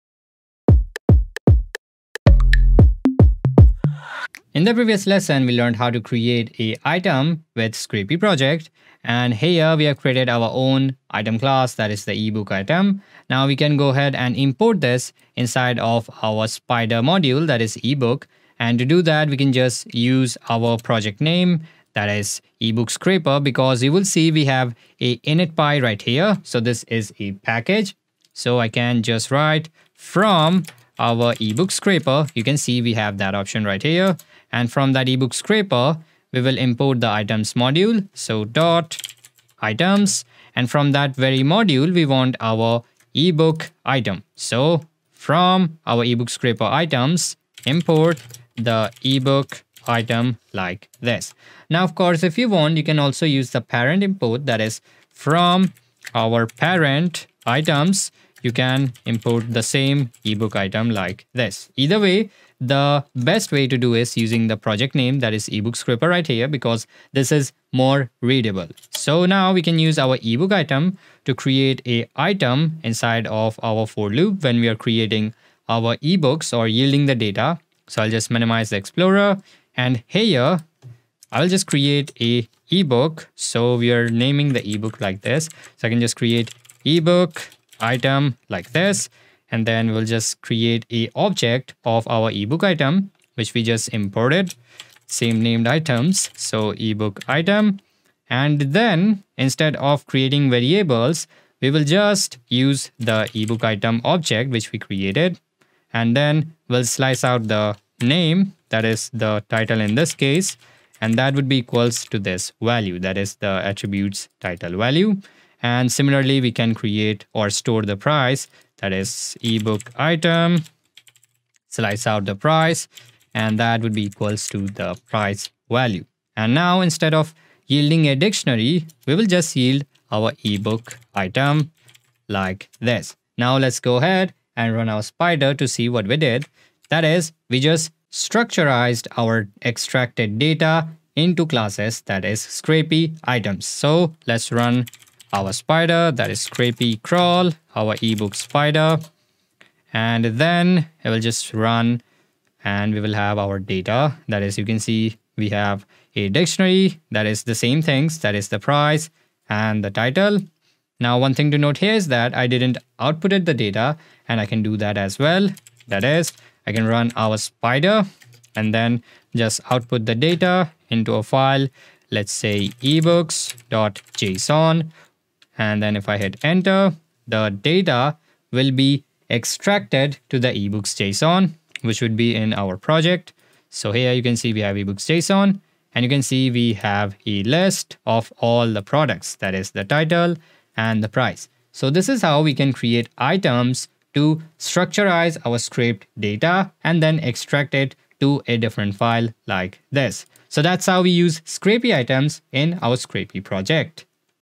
Scrapy Use Items - Storing Data in Items Feilds
Web scraping is the process of scraping or extracting data from websites using programs or other tools. And in this video lesson, we will learn how to populate data in items.

Full Course ✨ ✨ Web Scraping Masterclass

The main goal of scraping is to extract structured data from unstructured sources, typically, web pages. Spiders may return the extracted data as items, Python objects that define key-value pairs.

Scrapy supports multiple types of items. When you create an item, you may use whichever type of item you want. When you write code that receives an item, your code should work for any item type.

⏭️ #25 Scrapy Item Processors [ ... ]

🎵 Intro Music
Music from Uppbeat (free for Creators!):